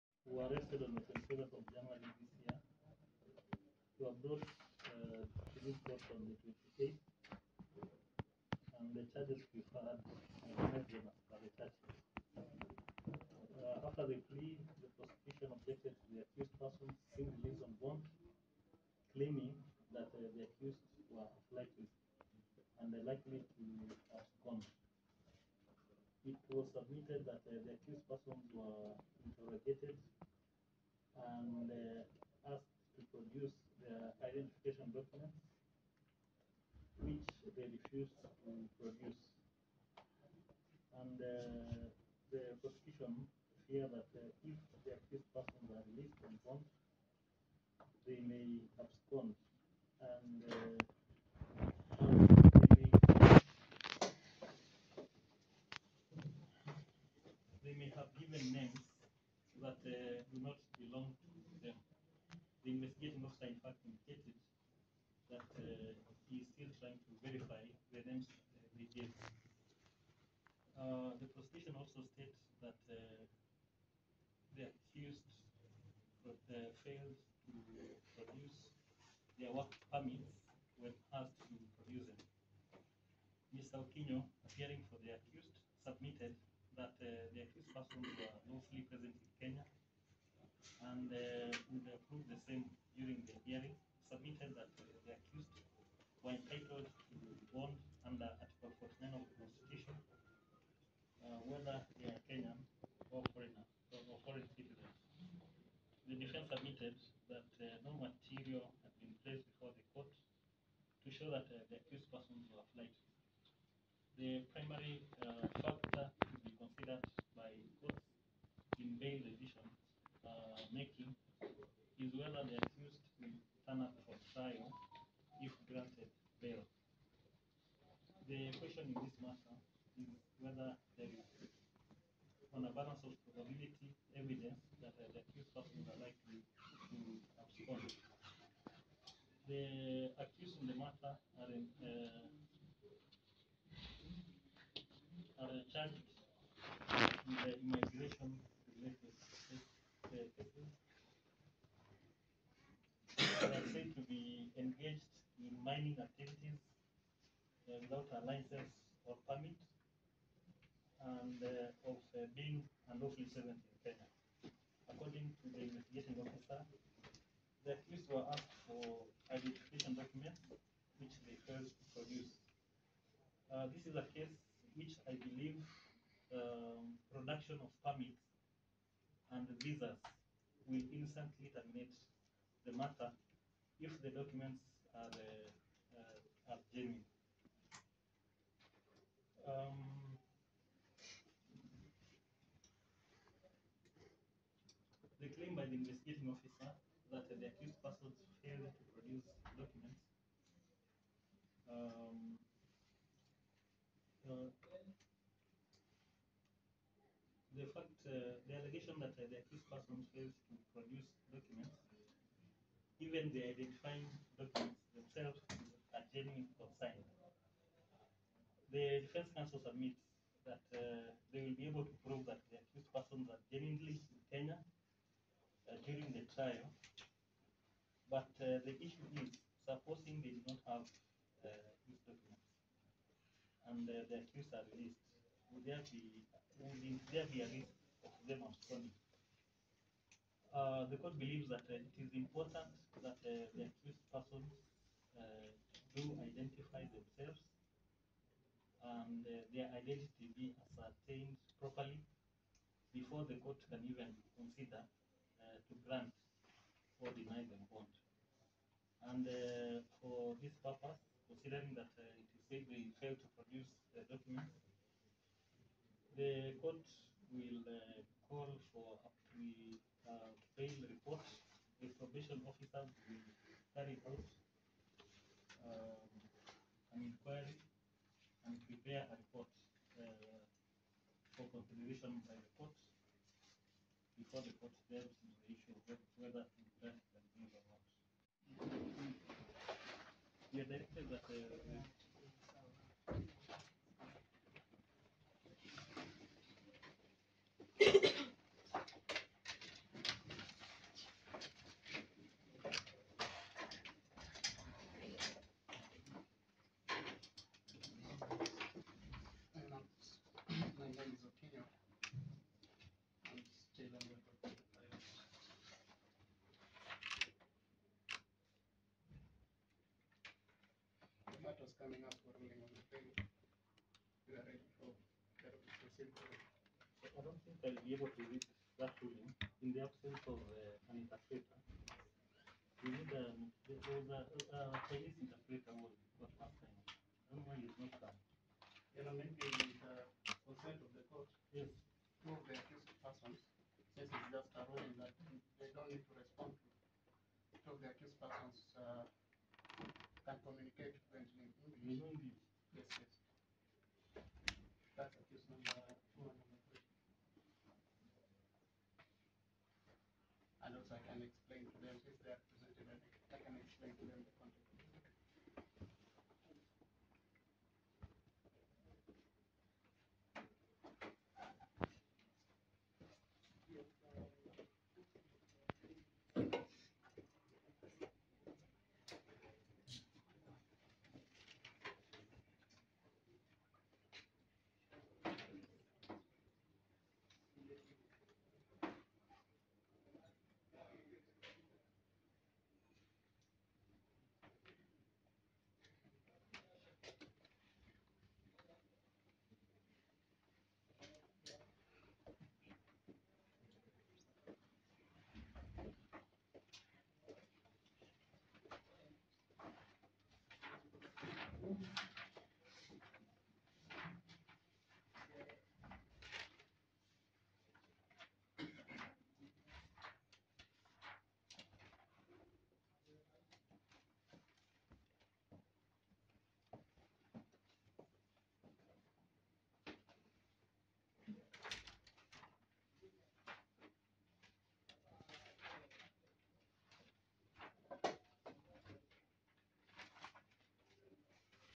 FIVE CHINESE CHARGED WITH ENGAGING IN ILLEGAL MINING IN KENYA TO REMAIN IN CUSTODY.
Five Chinese charged before a Nairobi Court with engaging in mining activities without license and engaging in business without authority to remain in custody. Milimani Senior Principal Magistrate Benard Ochoi declined to release Yao Youming, Edion Lion, Wangizi Ling, Shun Ming and Wang Yang on bail